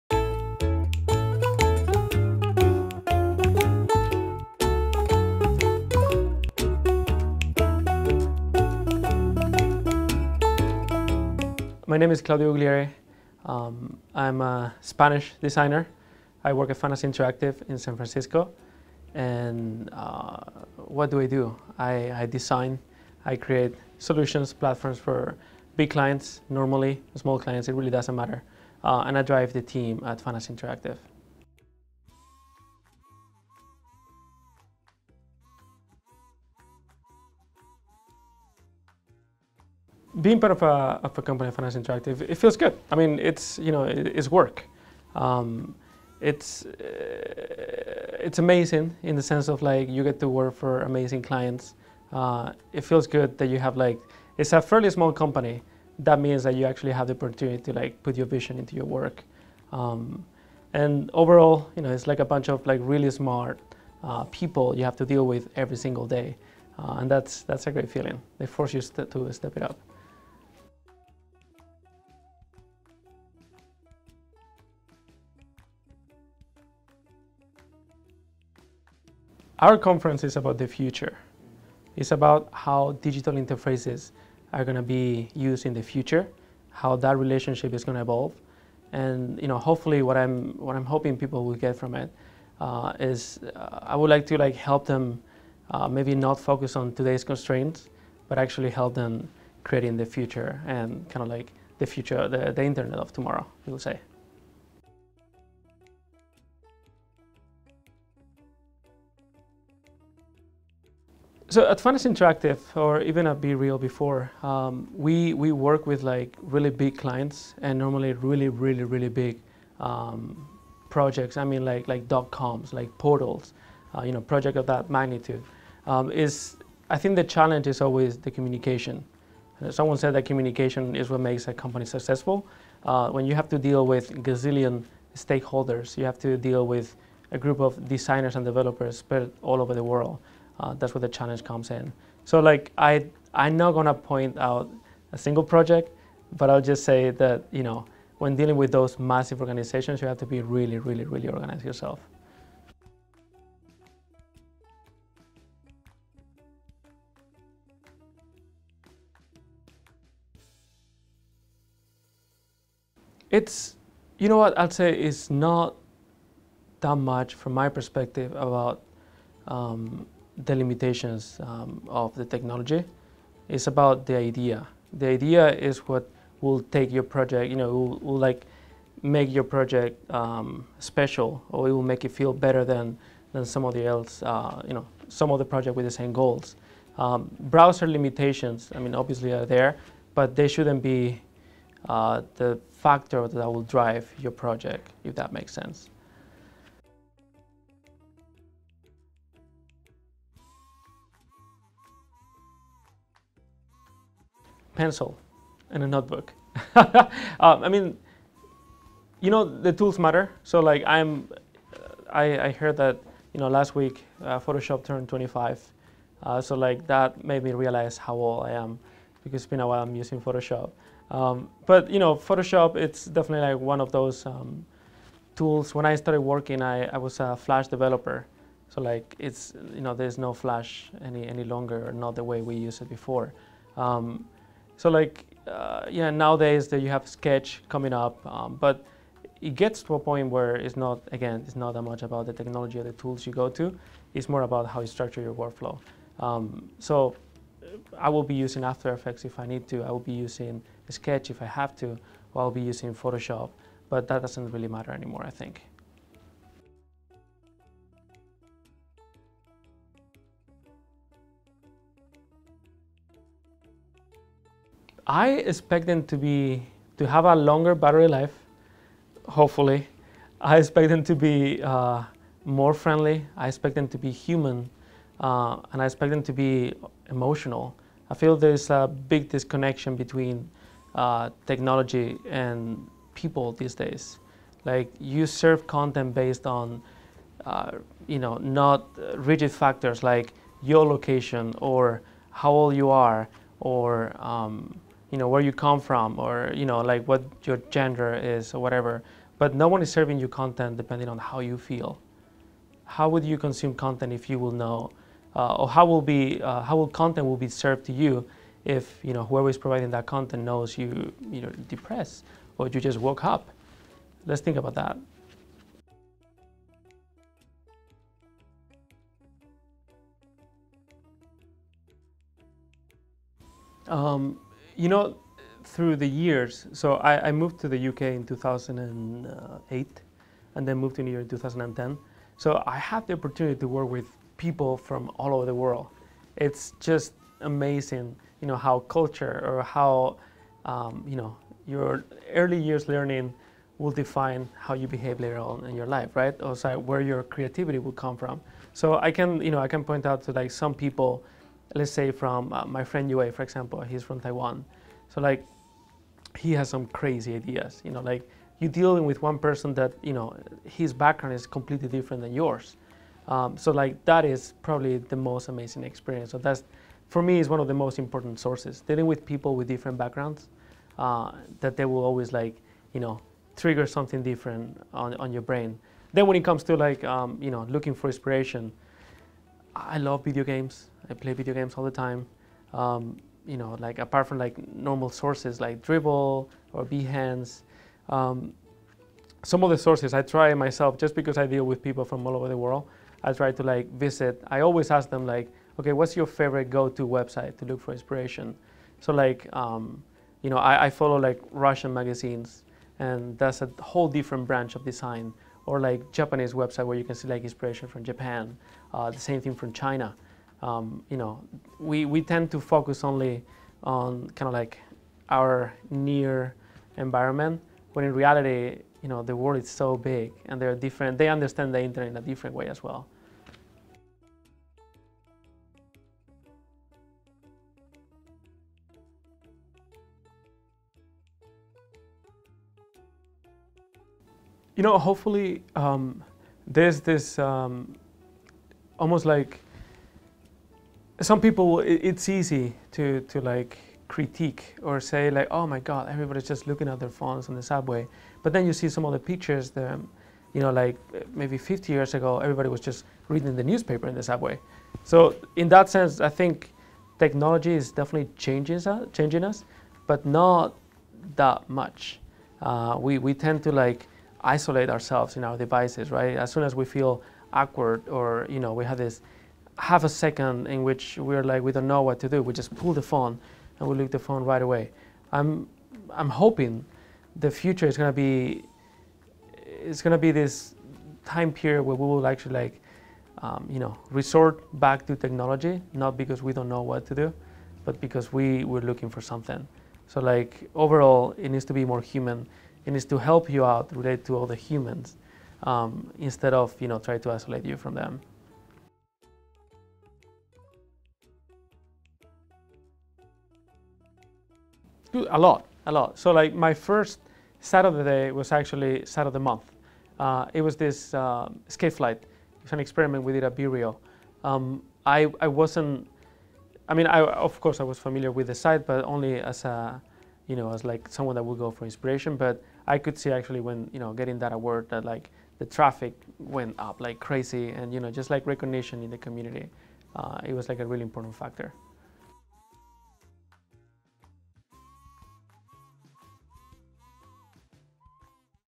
Claudio Guglieri on Digital Interfaces and Experimental Web Technologies
Claudio Guglieri, Design Director at Fantasy Interactive, talks Digital Interfaces, Experimental Web Technologies and the Internet of Tomorrow